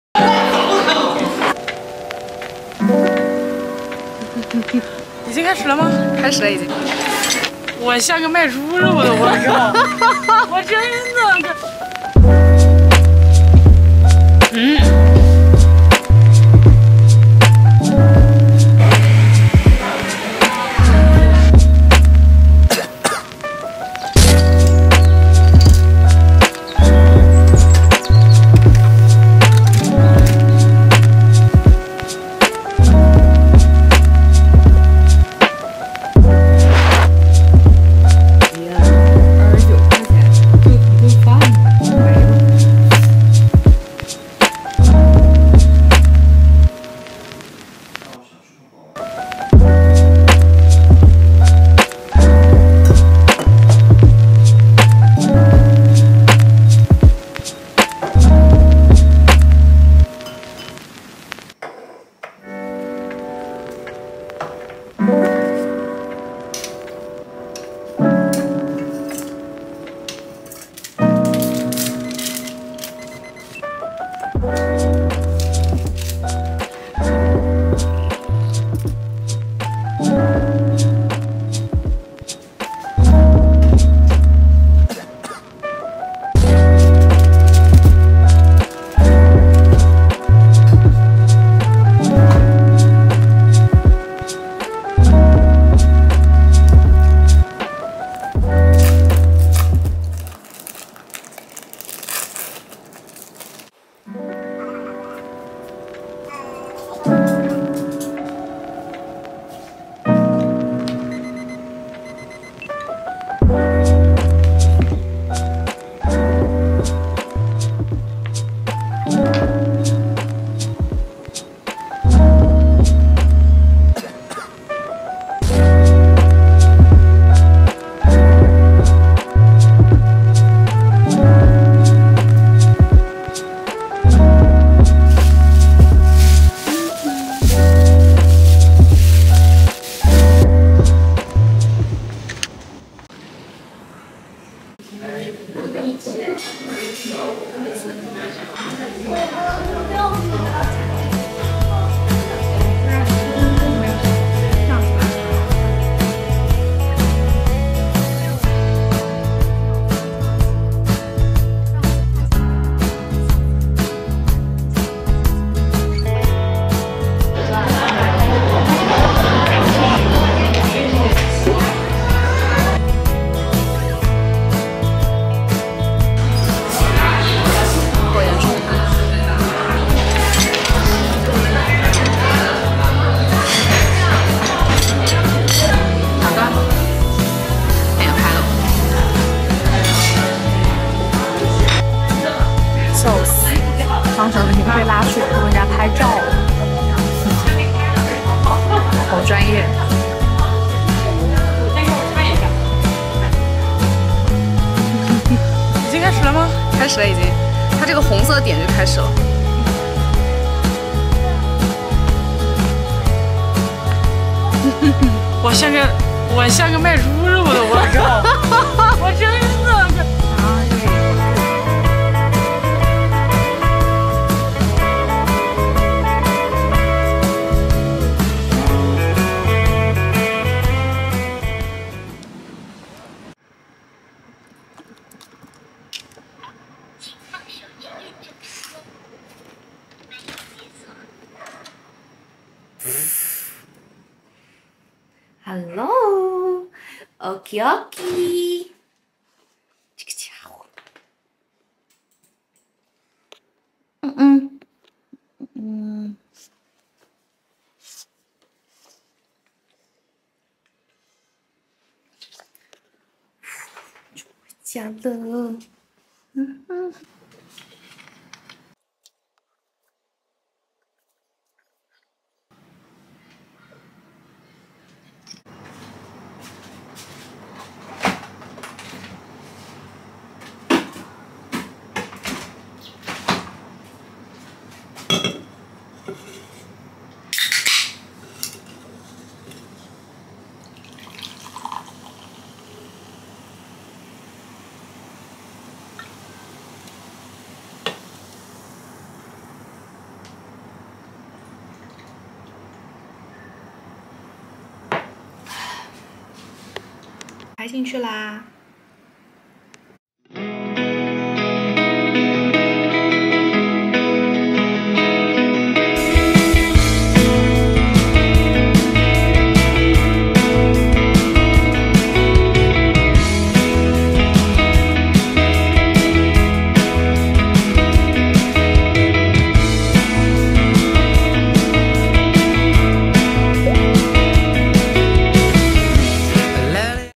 A day of my life/New coffee shop ticking
Easy Monday-hang out with friends/drink coffee/vlog/daily life